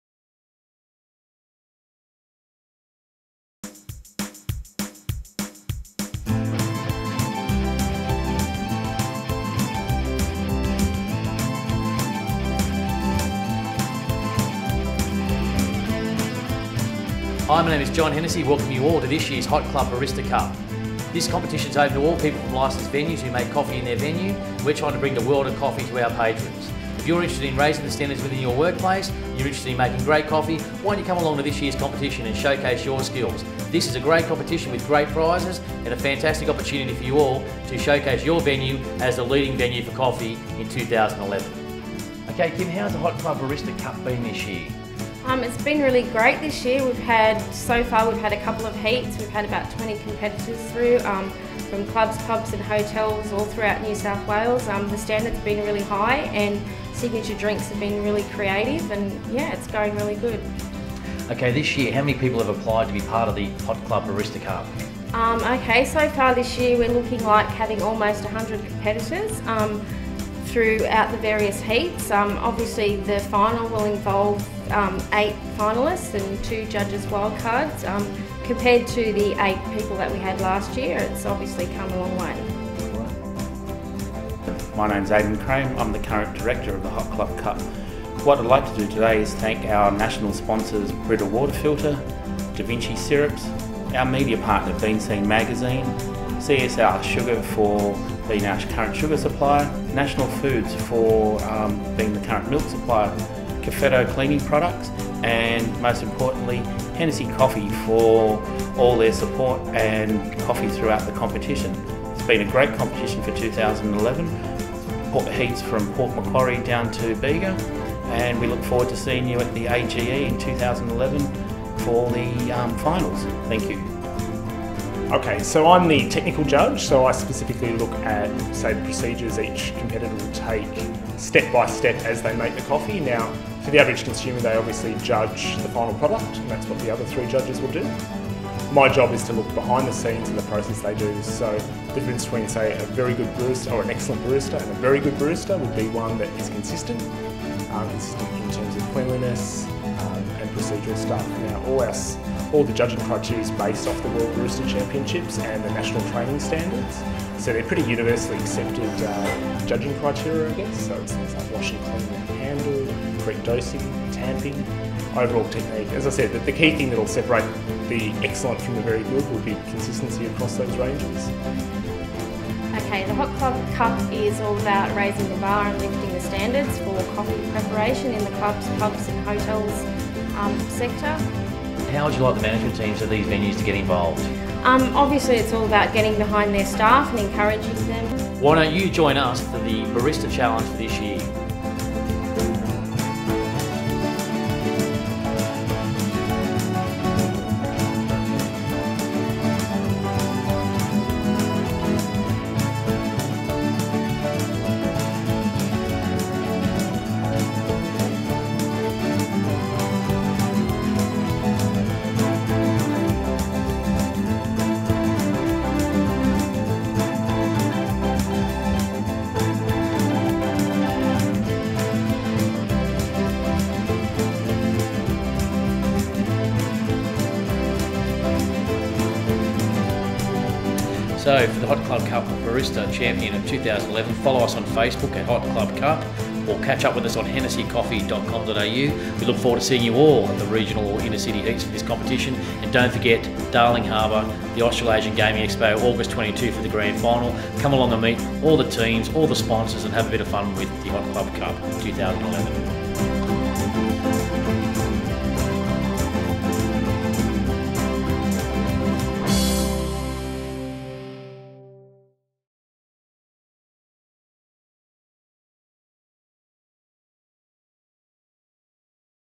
The Brita Professional HotClub Cup 2011
Heats from The Brita Professional HotClub Barista Cup, Australia's only specialty coffee competition aimed at the Hotels, Clubs and Pubs sector.

In just two years, the BRITA Professional HotClub Cup has made a name for itself in the Club & Hotel industry as a tough competition which lifts the bar for Baristas from this niche market, giving them the opportunity to showcase their skills and delight the judges.  "We have been so thrilled at the professionalism shown by all the entrants in this year's event," said John Hennessy, founder of the high-profile event. "The other judges and I have been especially impressed with the signature coffees where the entrants have shown amazing levels of creativity and flair."